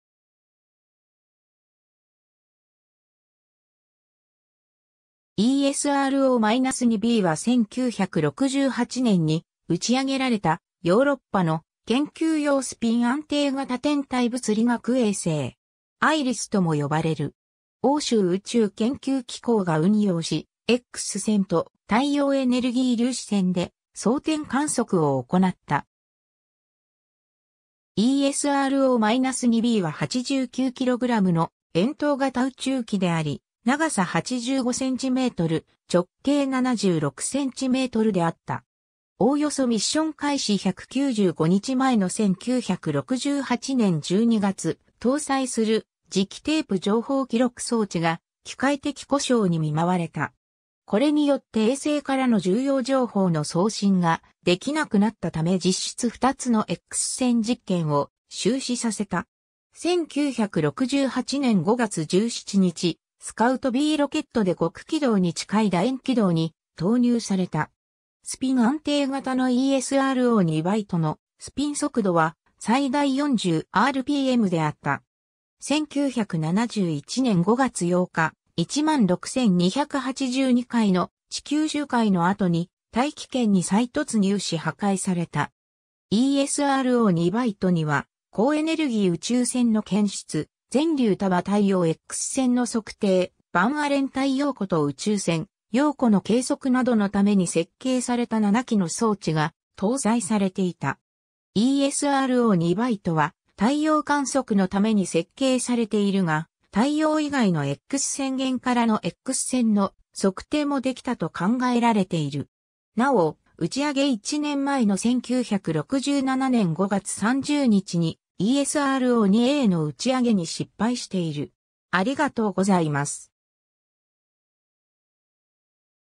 ESRO 2B
ESRO-2Bは1968年に打ち上げられたヨーロッパの研究用スピン安定型天体物理学衛星。
Iris (International Radiation Investigation Satellite、国際放射線調査衛星)とも呼ばれる。
 欧州宇宙研究機構(ESRO)が運用し、X線と太陽エネルギー粒子線で掃天観測を行った。
 ESRO-2Bは89kgの円筒型宇宙機であり、長さ85cm、直径76cmであった。
 おおよそミッション開始195日前の1968年12月、搭載する磁気テープ情報記録装置が機械的故障にみまわれた。
これによって衛星からの重要情報の送信が出来なくなったため実質2つのX線実験を終止させた。
 1968年5月17日、スカウトBロケットで極軌道に近い楕円軌道に投入された。
 スピン安定型のESRO 2Bのスピン速度は最大40rpmであった。
1971年5月8日、16282回の地球周回の後に大気圏に再突入し破壊された。
 ESRO 2Bには高エネルギー宇宙船の検出、全流束太陽X線の測定、ヴァン・アレン帯陽子と宇宙船陽子の計測などのために設計された7機の装置が搭載されていた。
ESRO 2Bは太陽観測のために設計されているが、太陽以外のX線源からのX線の測定も出来たと考えられている。
 ^ なお、打上げ一年前の1967年5月30日にESRO 2Aの打上げに失敗している